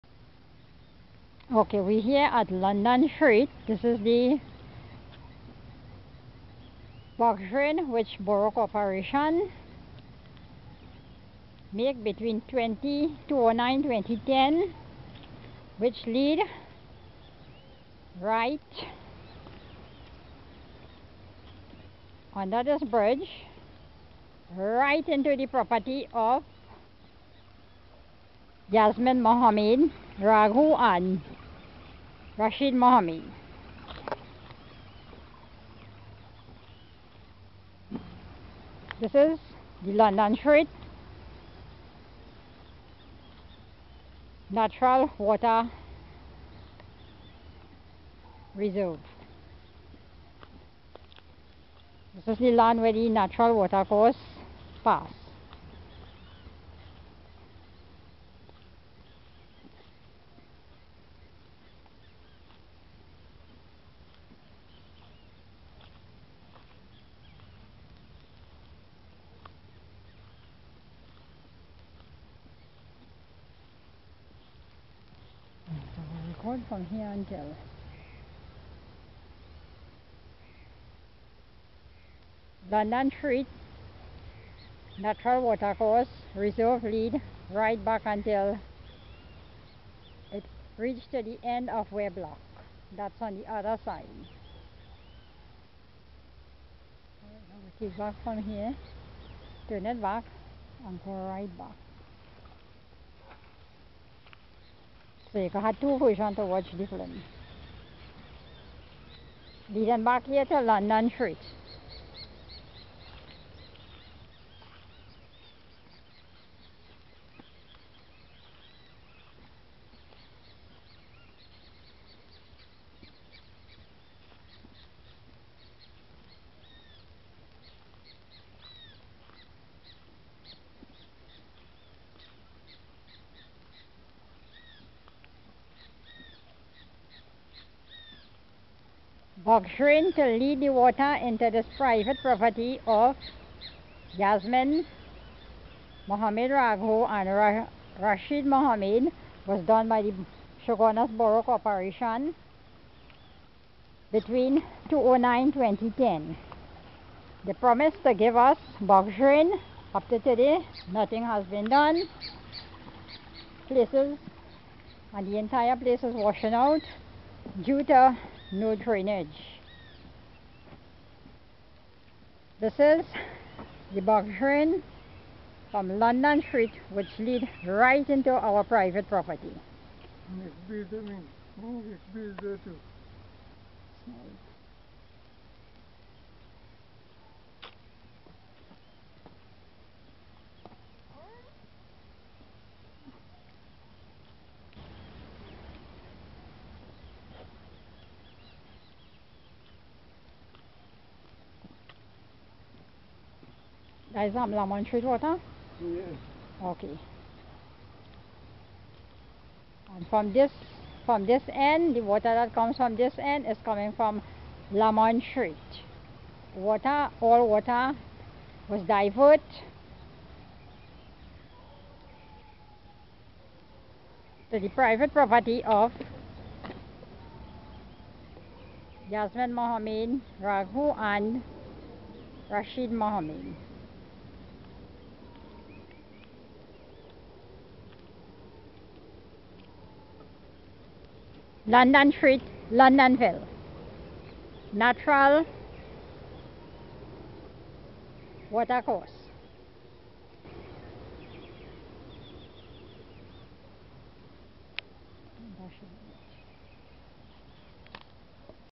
MOV09611 London Street, Longdenville,Chaguanas
The Main Natural Water Course which was also a decade ago, The Chaguanas Borough Corporation complete their Community Box drain which was lead right into our Family Private Property they promise us that the box drainage would be continued into our rice land (lagoon) Property back then, up to this day nothing has been done. We have mosquitoes infestation, Caiman out numbered, little children within the Community how safe are those children? Caiman had eaten Takram Sookraj. 2 pet dogs   How can the Chaguanas Borough Corporation, lead the entire Community waste water from London and Lamont Streets into a Private Property? Where do the Law stand here ? Its about time to get some legal advice? then lower down the land this man came put up a 4ft Wall in the middle of the reserve and back fill 350 loads of back fill material, now everyone are suffering with water log and viruses within the Community. We have written letters on the 5th of February 2014 to the Mayor Gopaul Boodan and Ceo Ashmead Mohammed of Chaguanas Borough Corporation, regarding the Natural Water Course within the land, nothing has been done meeting upon follow up meetings, and still nothing has been done, 9 months and going, we are still waiting on them, games should be over regarding citizen safety and health, all citizen health remain in the hands of mayor Gopaul Boodan and Ceo Ashmead Mohammed, should anyone get ill again, we had had chikungunya and the dengue, affected citizens from the streets as followed, London Street, Lamont Street, Bristol Street, Northy Lane and Railway Road, citizen are still recovering from the virus.We are calling upon our Area Representative Dr. Tim Gopeesingh, Health Minister Dr. Fuad Khan, please intervene help members of the Chaguanas Corporation to get the retaining wall break down, and run the box drain, or some cylinders, we have already declared the Natural Water in a Petition, and has an Agreement related to all Owners within the said Property give consent to have the drainage pass through their land. What more can the Chaguanas Borough Corporation ask for, all document was delivered to them. Would the Mayor Gopaul Boodan or Ceo Ashmead Mohammed find another place for the Community water to flow, since they are have not felt the interest to complete this drainage work.